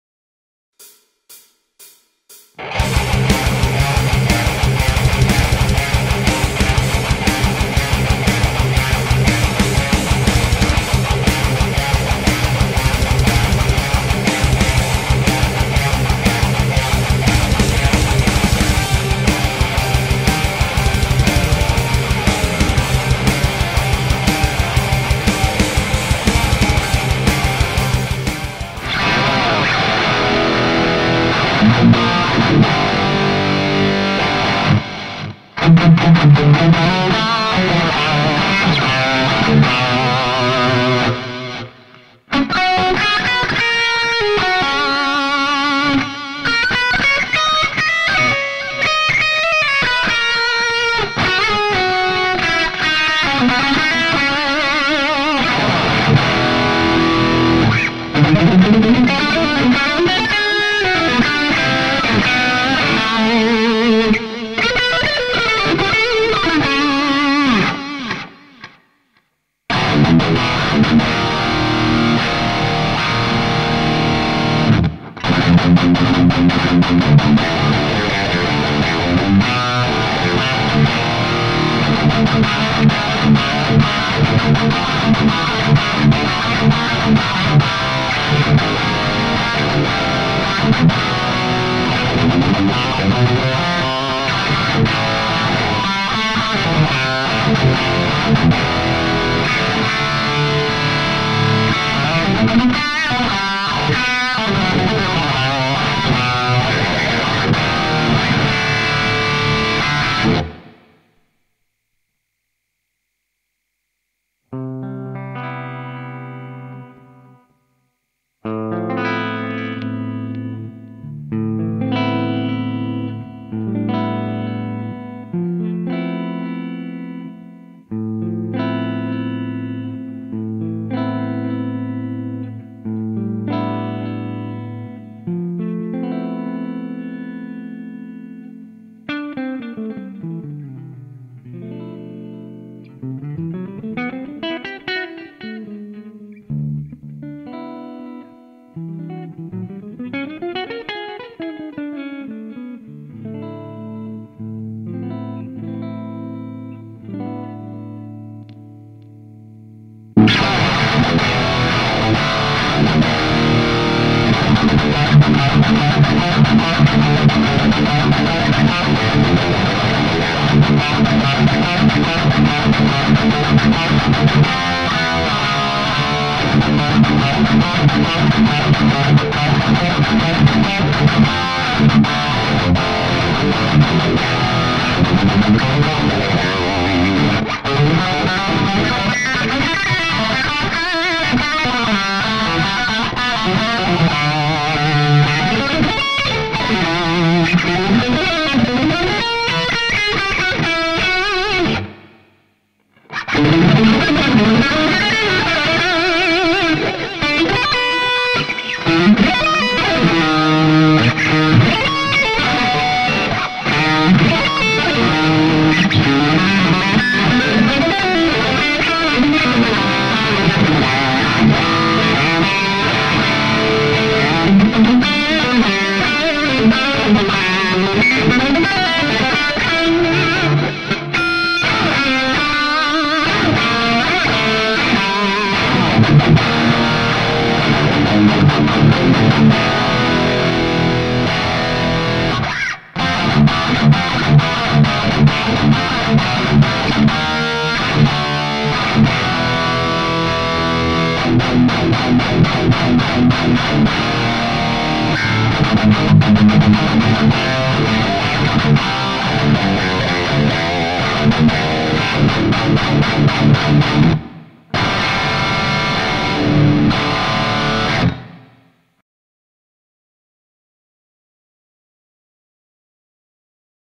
Solar A1.6C 1st impression
NGD day.

It been quite a while since got de quervain,wrist injury..

This thing is BEAST!!Sounds big!!n lot lot of gain..

1 of my best purchase of all time..

Thanks Ola for designing this..and yes this is 1 of best instrument ever built for metalheads!!salute!!